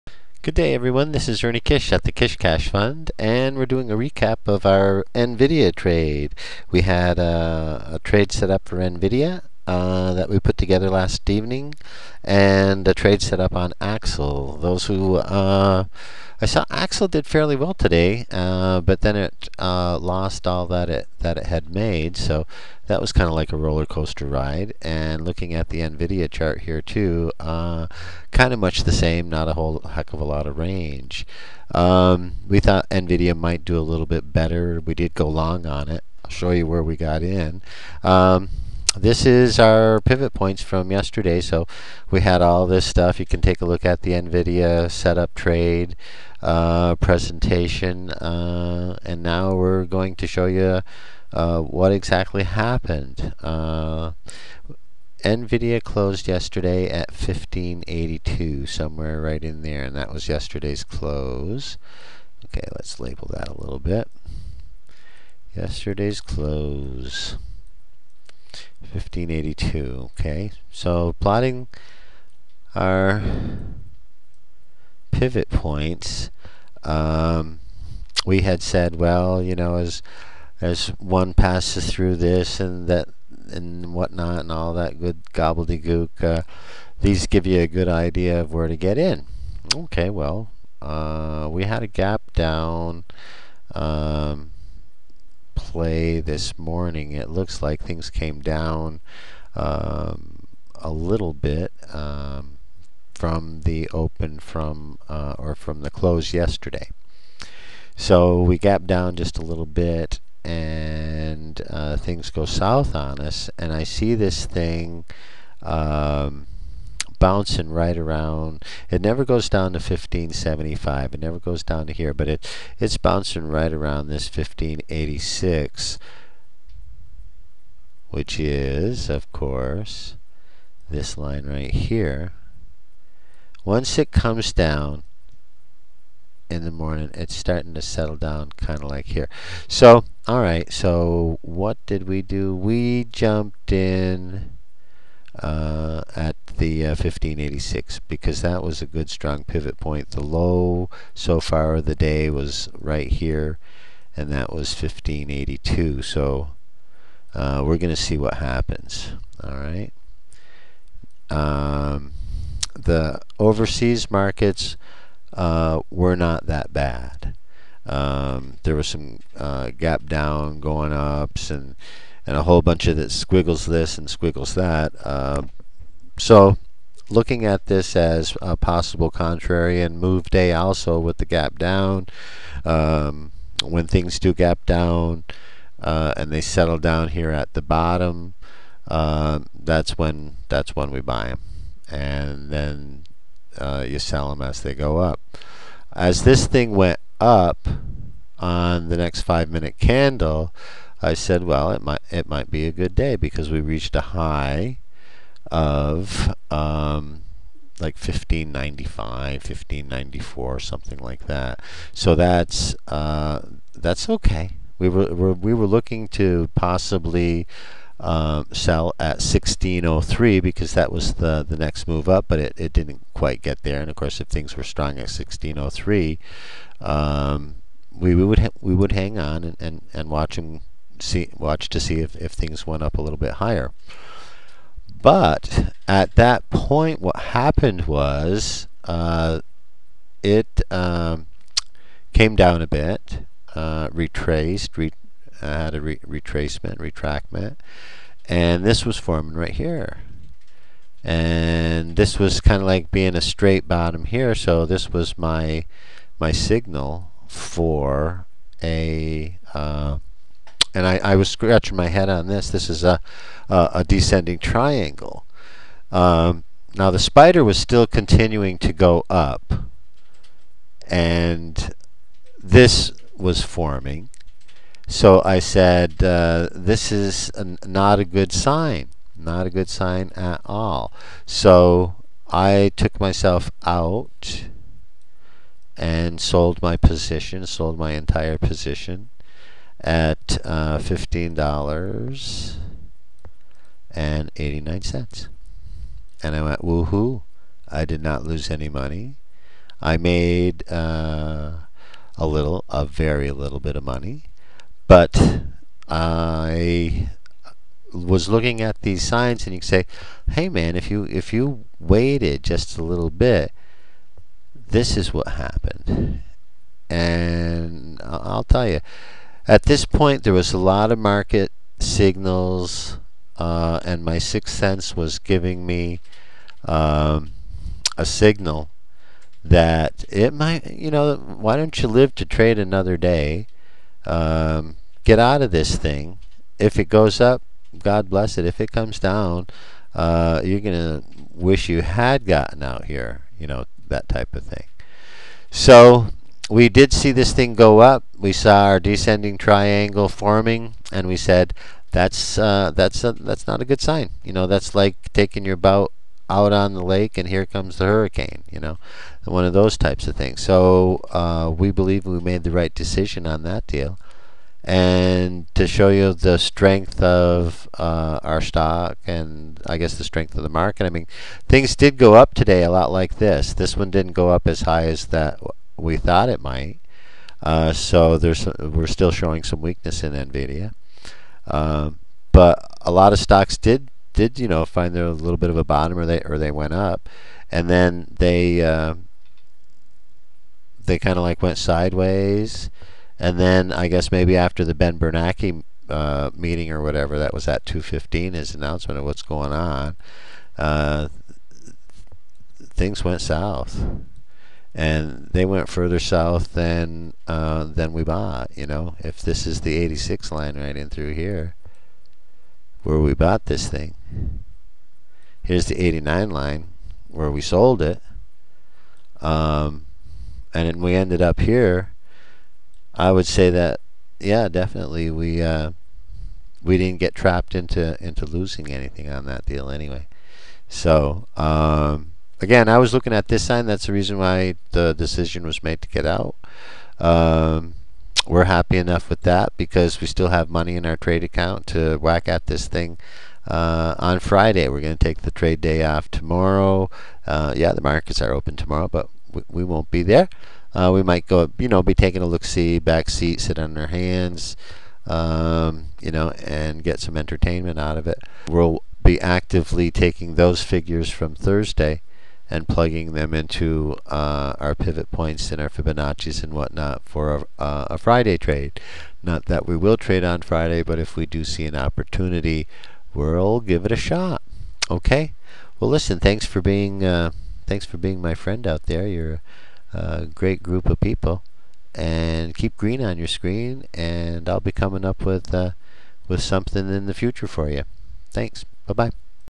NVDArecapfor6222011.avi
Trade recap of NVDA June 22, 2011